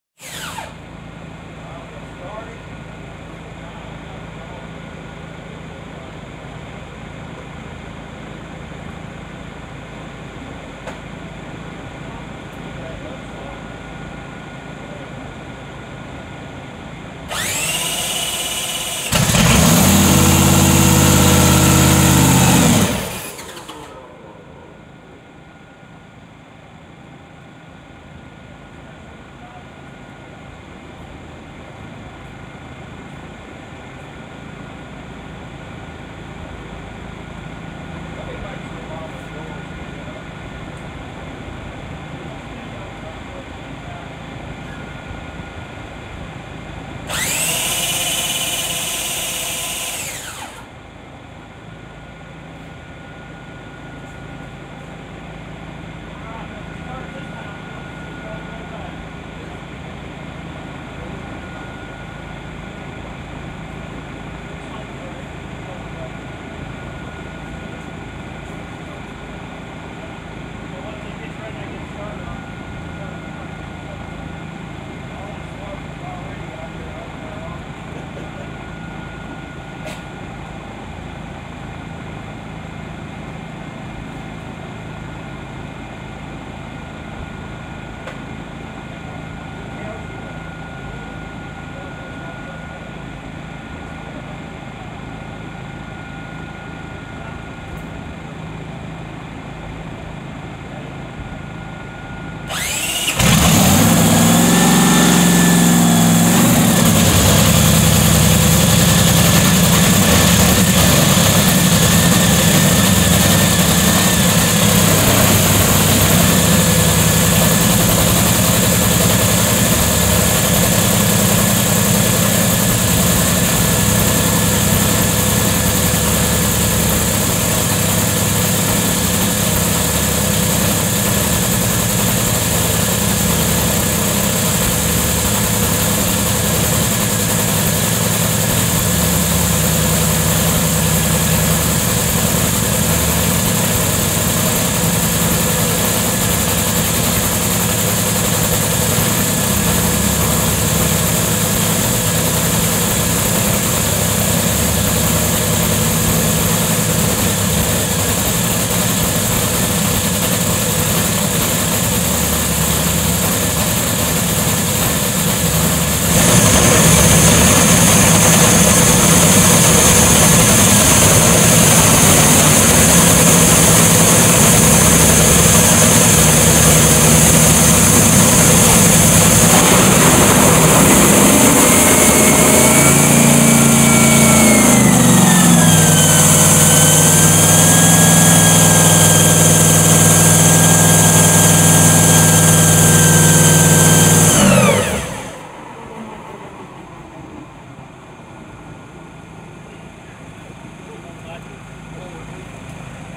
Scott Warming up the War Wagon
Scott Warming up the War Wagon world's fastest AA/FA 304.59 mph Out of Springtown Tx. Farris family. driven by Kevin Kinsley.